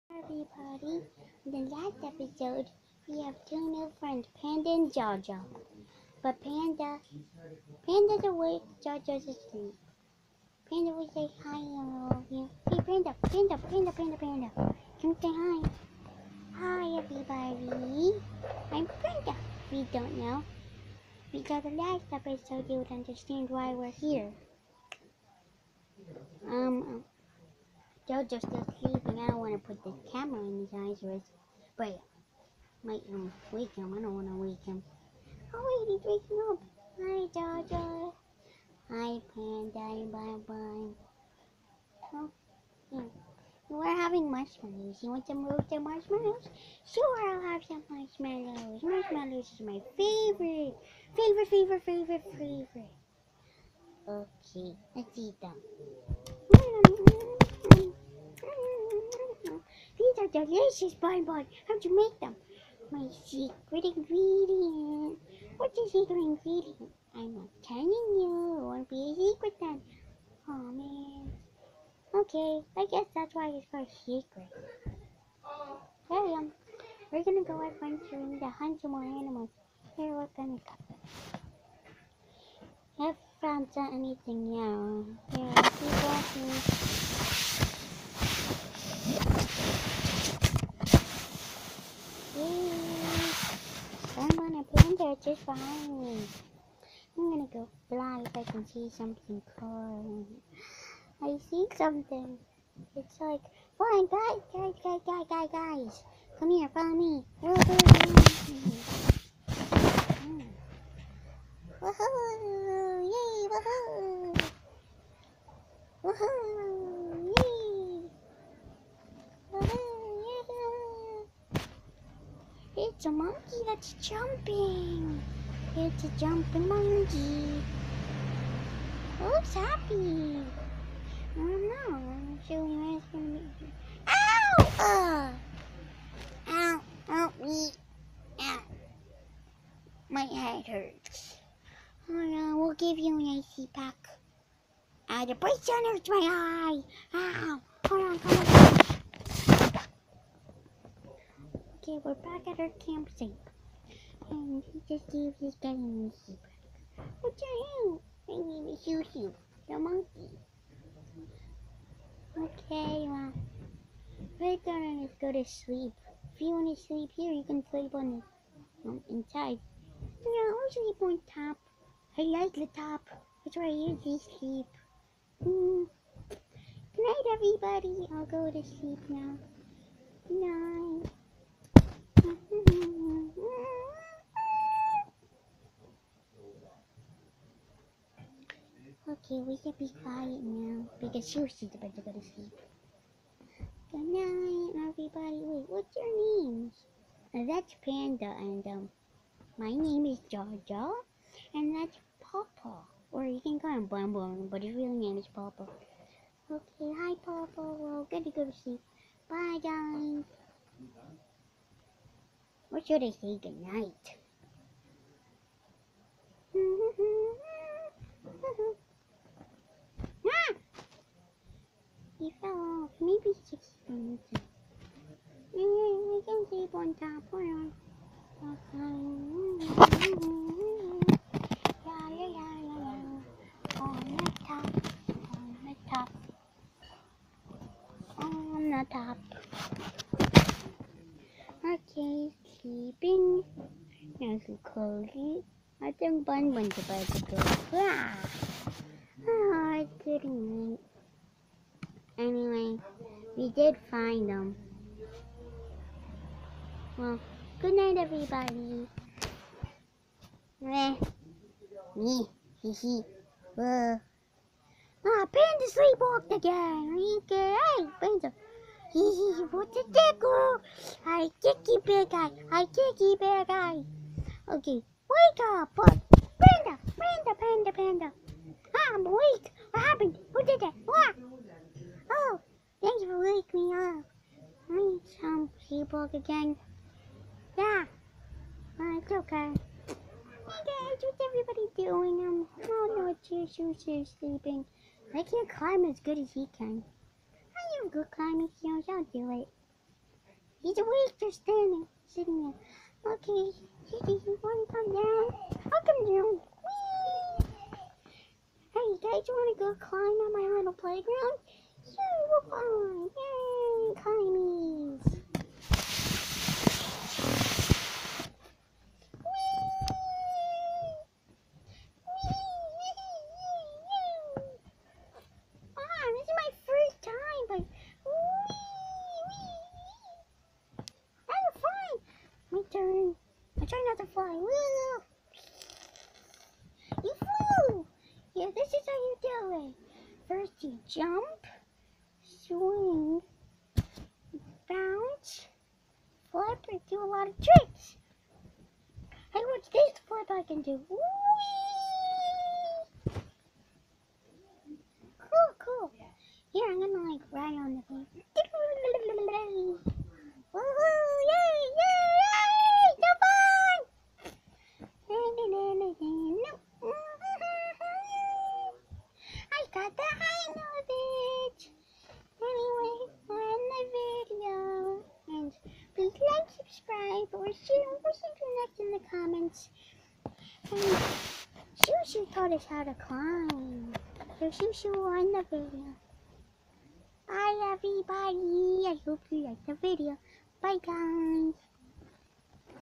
Bun bun's quest part 3! Climbing on trees with a new friend!!!!😃
I hope you like this video! Who is bun bun's, panda's, and jaw jaw's new friend!!?? Let's go climb into this adventure!!!